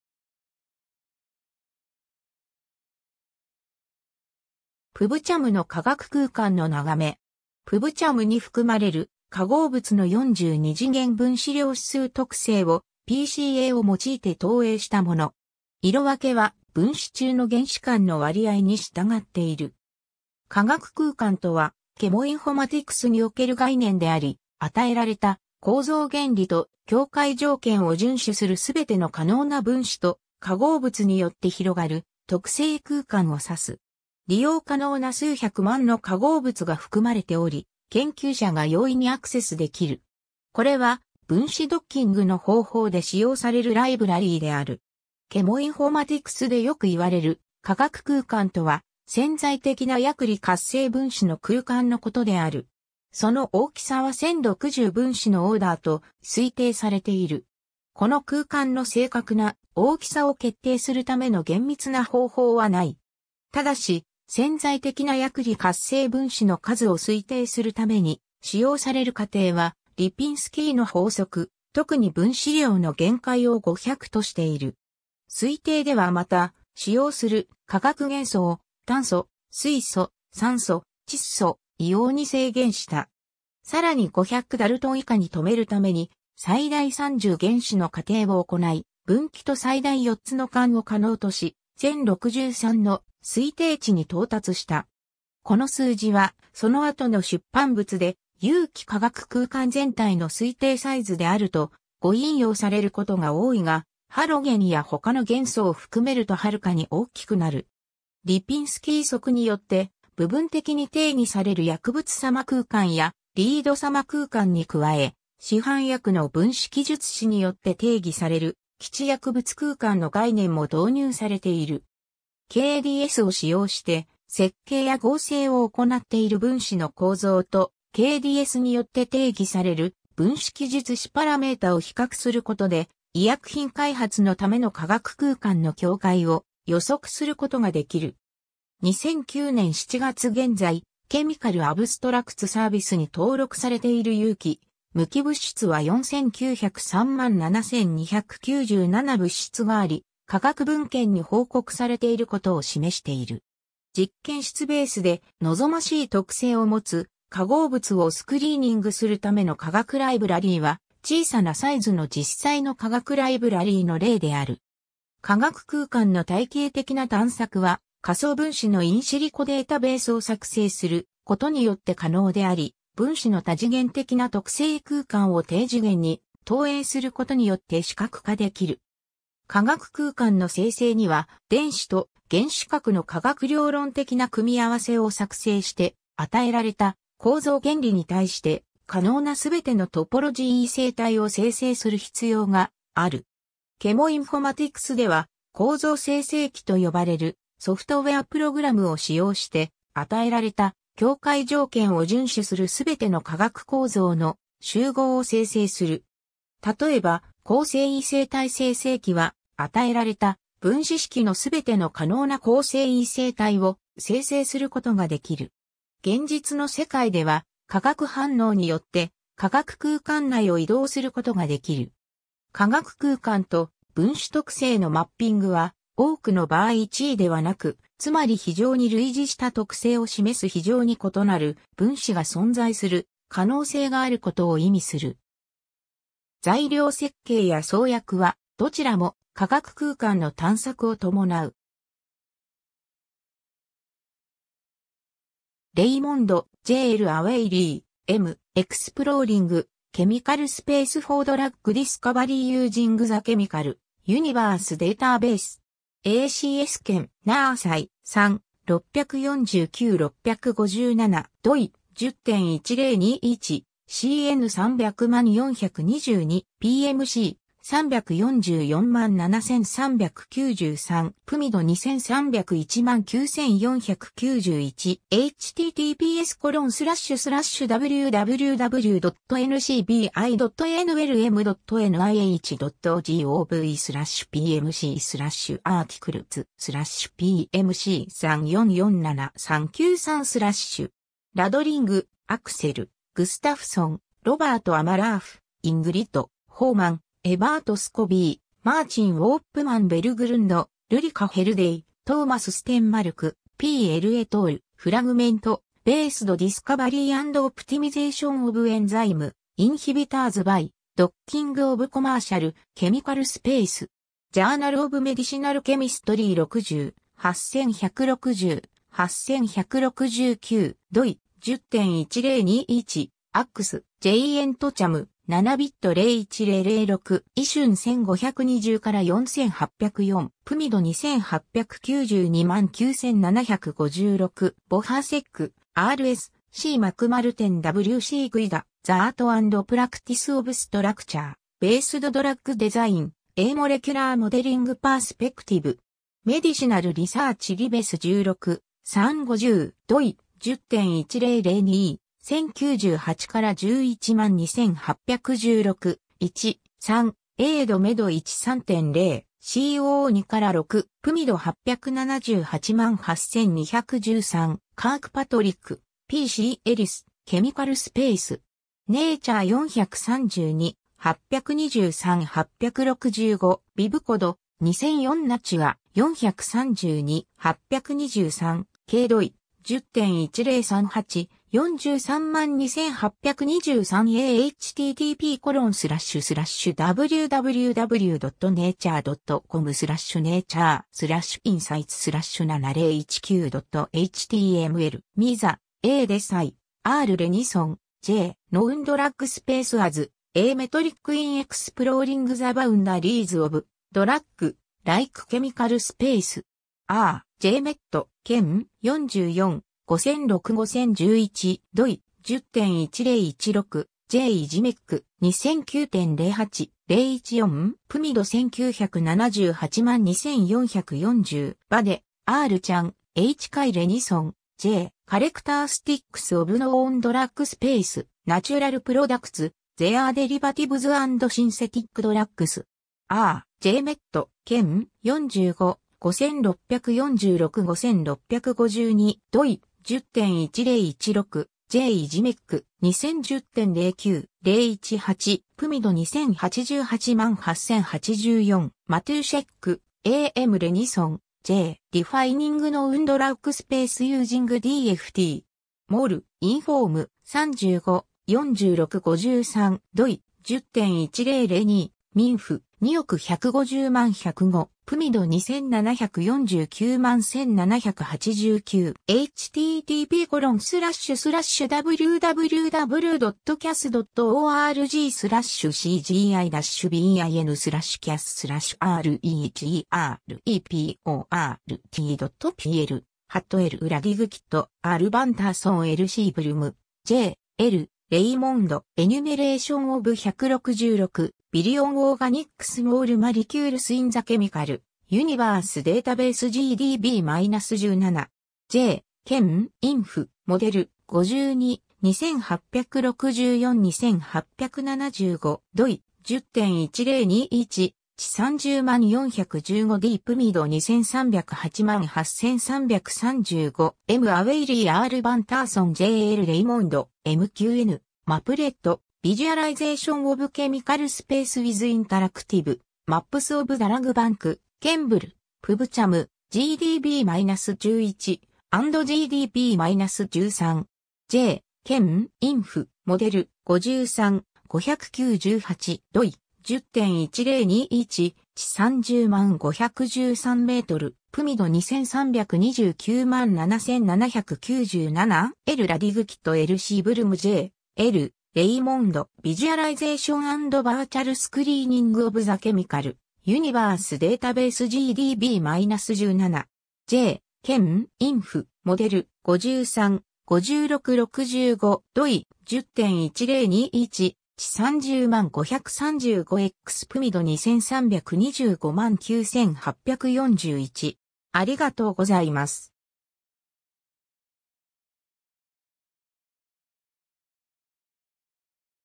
化学空間
PubChemの化学空間の眺め。
PubChemに含まれる化合物の42次元分子量子数 (MQN) 特性をPCAを用いて投影したもの (仮想的に作成された5つの化合物のライブラリ)。
色分けは分子中の原子環の割合 (青0、赤1) に従っている。
 化学空間 (英語: chemical space)とは、ケモインフォマティクスにおける概念であり、与えられた構造原理と境界条件を遵守するすべての可能な分子と化合物によって広がる特性空間を指す。
利用可能な数百万の化合物が含まれており、研究者が容易にアクセスできる。
これは、分子ドッキングの方法で使用されるライブラリである。
 ケモインフォマティクスでよく言われる化学空間とは、潜在的な薬理活性分子の空間のことである。
その大きさは1060分子のオーダーと推定されている。
この空間の正確な大きさを決定するための厳密な方法はない。
ただし、潜在的な薬理活性分子の数を推定するために使用される仮定は、リピンスキーの法則、特に分子量の限界を500としている。
推定ではまた、使用する化学元素を炭素、水素、酸素、窒素、硫黄に制限した。
さらに500ダルトン以下に止めるために最大30原子の仮定を行い、分岐と最大4つの環 (芳香族性(英語版)) を可能とし、1063の推定値に到達した。
この数字は、その後の出版物で有機化学空間全体の推定サイズであると誤引用されることが多いが、ハロゲンや他の元素を含めるとはるかに大きくなる。
リピンスキー則によって部分的に定義される薬物様空間やリード様空間に加え、市販薬の分子記述子によって定義される既知薬物空間 (英語: known drug space; KDS) の概念も導入されている。
KDSを使用して、設計や合成を行っている分子の構造と、KDSによって定義される分子記述子パラメータを比較することで、医薬品開発のための化学空間の境界を予測することができる。
 2009年7月現在、Chemical Abstracts Serviceに登録されている有機・無機物質は49,037,297物質があり、科学文献に報告されていることを示している。
実験室ベースで望ましい特性を持つ化合物をスクリーニングするための化学ライブラリは、小さなサイズ (数十万～数十万分子) の実際の化学ライブラリの例である。
 化学空間の体系的な探索は、仮想分子のインシリコデータベース(英語: in silico database)を作成することによって可能であり、分子の多次元的な特性空間を低次元に投影することによって視覚化できる。
化学空間の生成には、電子と原子核の化学量論的な組み合わせを作成して、与えられた構造原理に対して可能なすべてのトポロジー異性体を生成する必要がある。
ケモインフォマティクスでは、構造生成器(英語: Structure Generators)と呼ばれるソフトウェアプログラムを使用して、与えられた境界条件を遵守するすべての化学構造の集合を生成する。
たとえば、構成異性体生成器(英語: Constitutional Isomer Generators)は、与えられた分子式 (元素の割合を示すグロス式) のすべての可能な構成異性体を生成することができる。
 現実の世界では、化学反応によって化学空間内を移動することができる。
化学空間と分子特性のマッピングは多くの場合一意ではなく、つまり非常に類似した特性を示す非常に異なる分子が存在する可能性があることを意味する。
材料設計や創薬は、どちらも化学空間の探索を伴う。
^ Reymond, J.-L.; Awale, M. (2012). “Exploring chemical space for drug discovery using the chemical universe database.”. ACS Chem. Neurosci. 3 (9): 649–657. doi:10.1021/cn3000422. PMC: 3447393. PMID 23019491. ^ Rudling, Axel; Gustafsson, Robert; Almlöf, Ingrid; Homan, Evert; Scobie, Martin; Warpman Berglund, Ulrika; Helleday, Thomas; Stenma...